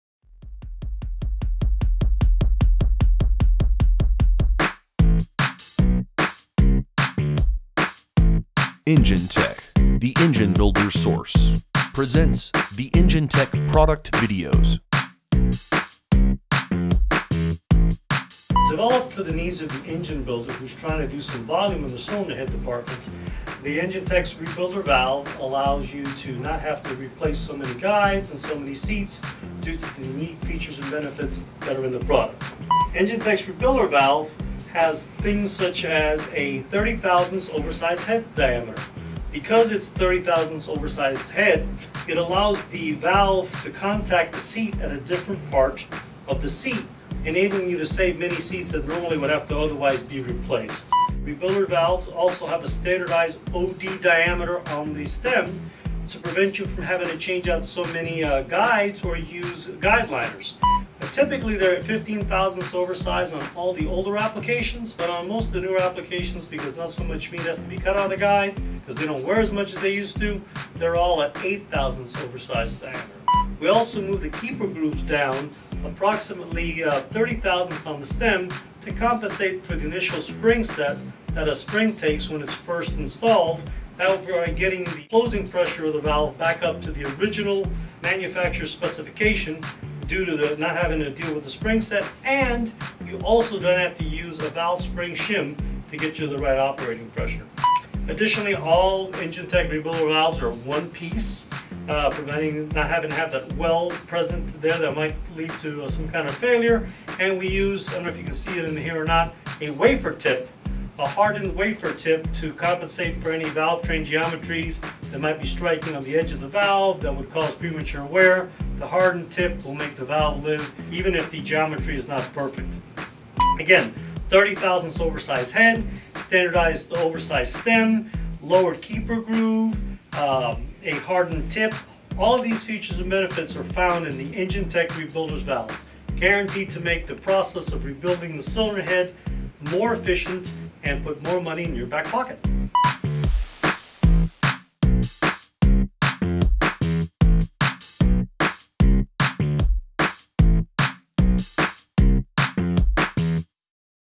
Rebuilders Valves - Enginetech
- Enginetech - The Engine Builder's Source

The Enginetech patented "Rebuilder's Valve®" is designed to open up that bottle neck in your cylinder head department. For about the same cost of reclaiming junk valves you can install brand new Rebuilder's Valves®. Rebuilders can increase cylinder head sales by marketing their remanufactured heads with "all new premium valves" compared to the competition that still uses "reclaimed junk valves".